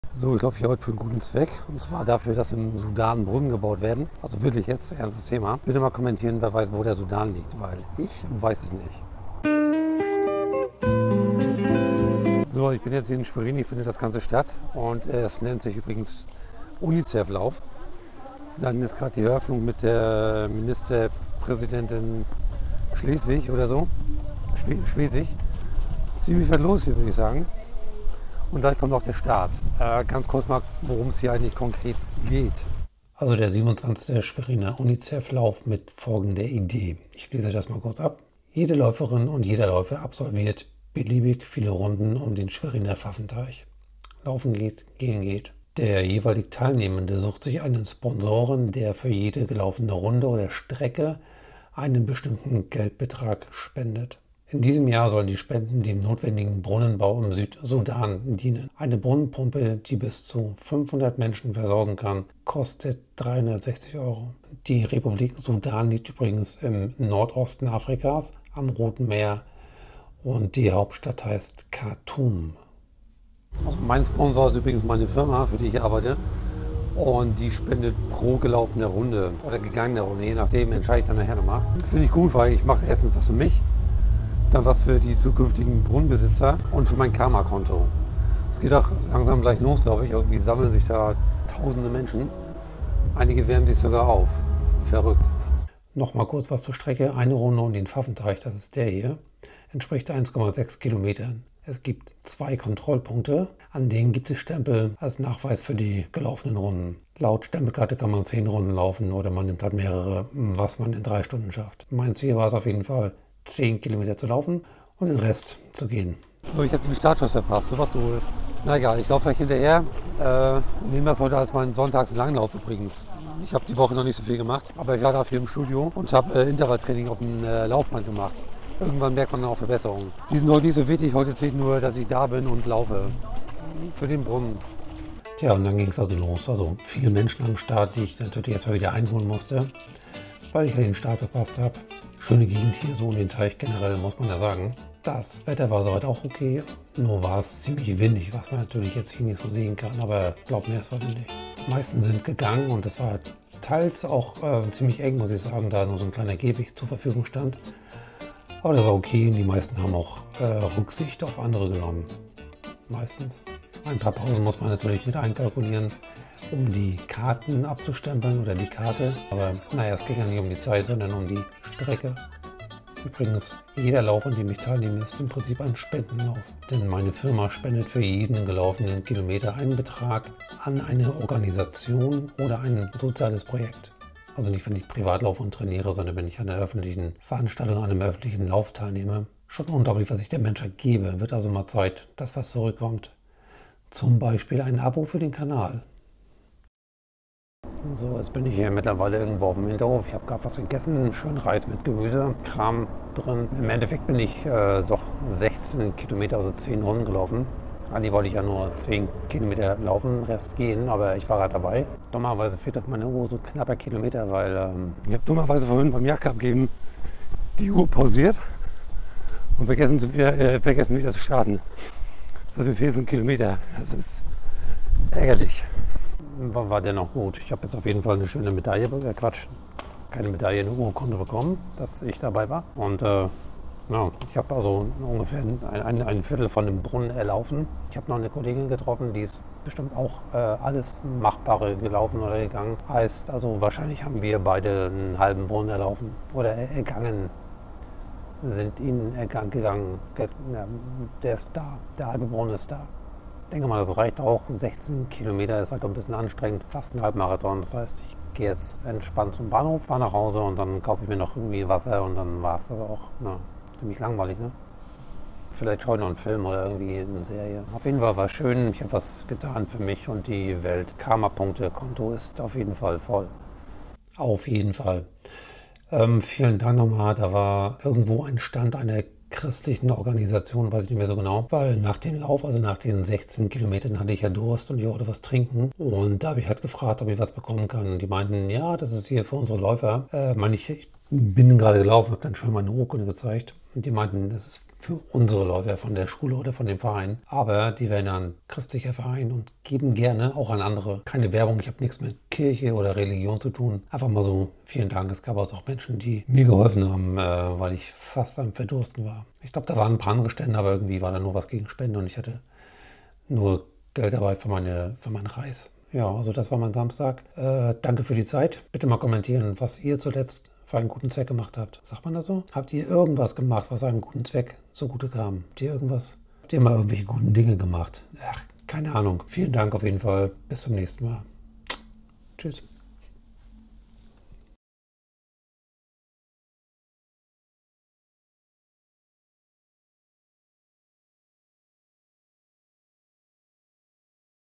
Kein Wasser in den Beinen – Laufen für Brunnen beim 27. UNICEF-Lauf in Schwerin
Ich bin am 14. September für einen sehr guten Zweck gelaufen - nämlich für einen Brunnenbau im Sudan.

Ciao,
Stephan